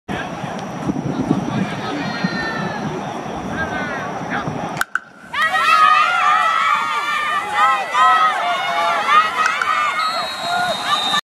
神戸大学 vs 関西大学 プレー動画No.6（2019年9月7日 EXPO FLASH FIELD）
神戸大学Ravens vs 関西大学Kaisers
2019年9月7日 ＠EXPO FLASH FIELD
2Q 神戸大学#19是澤太朗から#27森分優人への13ydパス